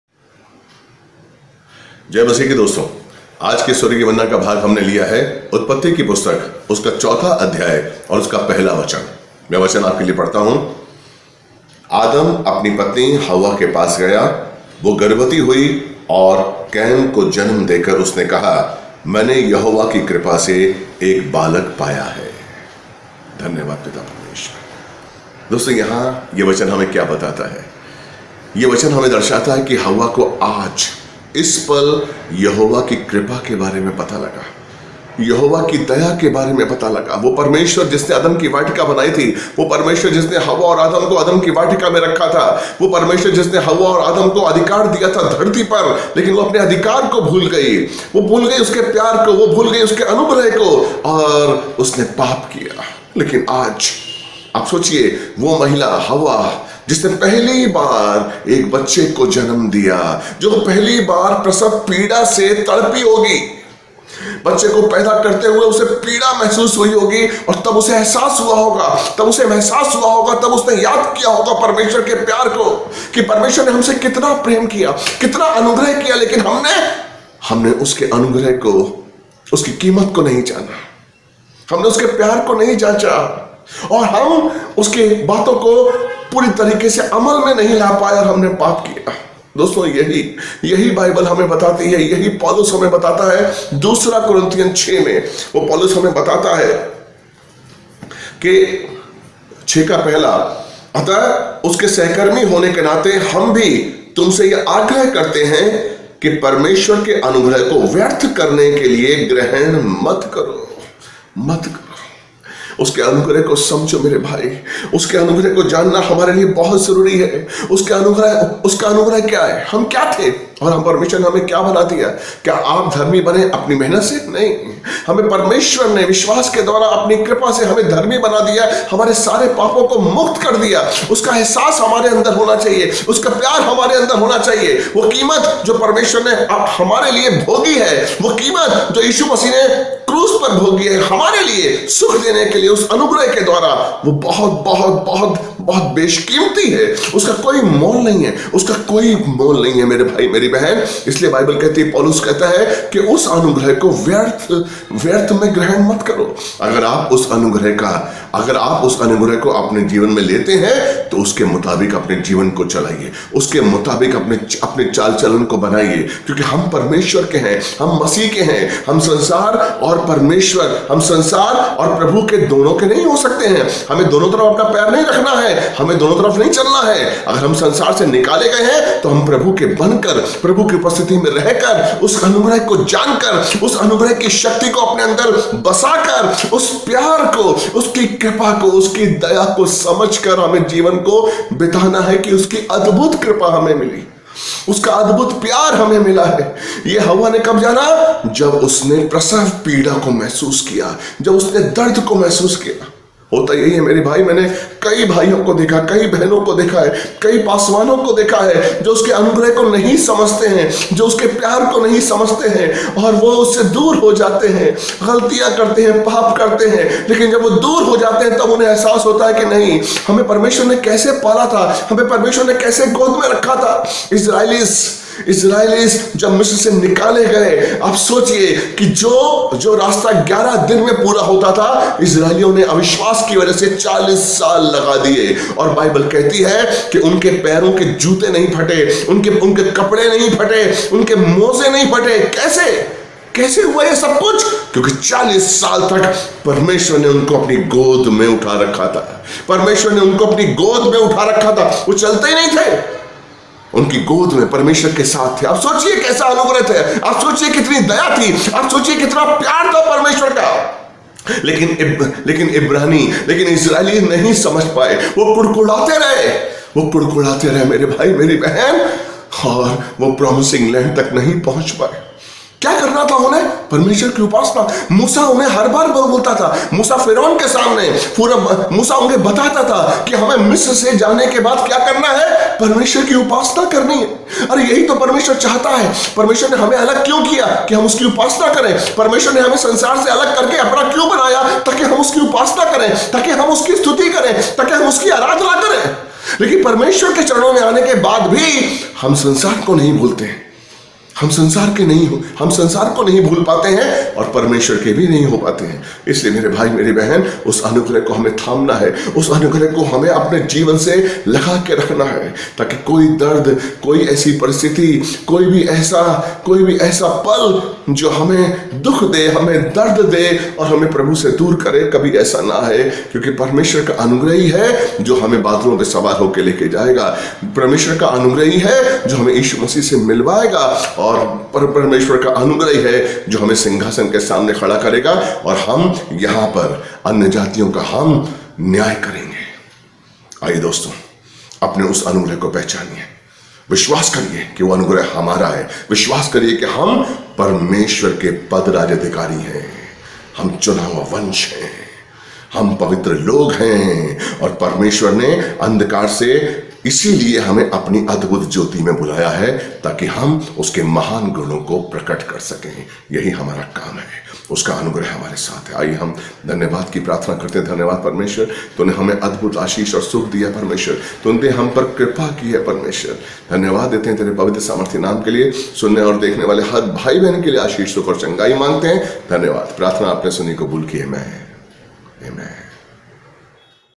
321 Heavenly Manna - Anugrah ki kimat ko jano (Pastor Anil Kochar)
उत्पत्ति- ४:१
आदम अपनी पत्नी हव्वा के पास गया वह गर्भवती हुई और कैन को जन्म देकर उसने कहा, मैं ने यहोवा की कृपा से एक बालक पाया है।
2 कुरिन्थियों 6:1
हम जो उसके सहकर्मी हैं यह भी समझाते हैं, कि परमेश्वर का अनुग्रह जो तुम पर हुआ, व्यर्थ न रहने दो।
स्वर्गीय मन्ना - पास्टर अनिल कोचर